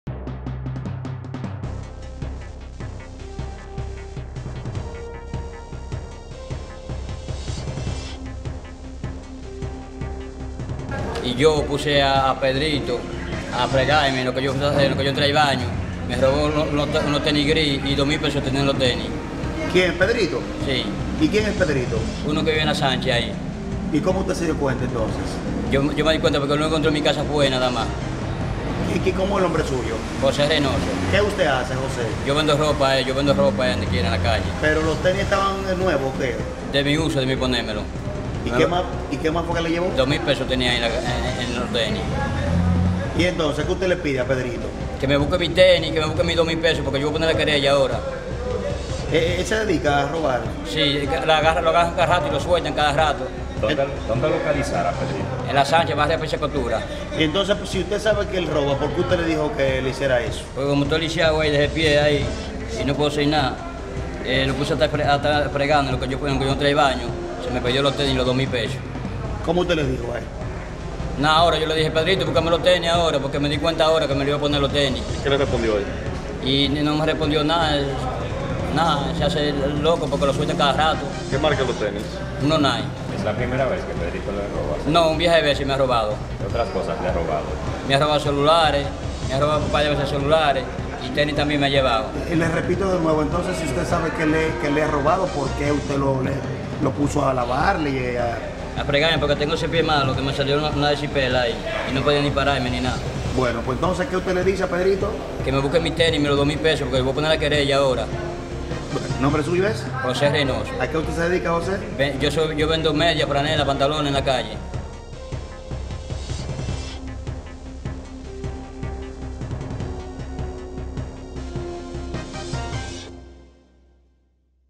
Hombre dice un vecino suyo le robó calzados y dinero en SFM
San Francisco de Macorís.- Un hombre denunció que un vecino suyo le sustrajo piezas de calzado y dinero en efectivo, en un hecho ocurrido en el barrio Pisacostura.

Según José Reynoso, el hecho sucedió cuando el presunto ladrón identificado como “Pedrito” estaba en su casa ubicada en la callé Sánchez número 13, fregando unos platos y aprovechó el robo mientras hacia sus necesidades.

El denunciante indicó que no es la primera vez que el acusado le roba, por lo que le exigió que le busque sus pertenencias.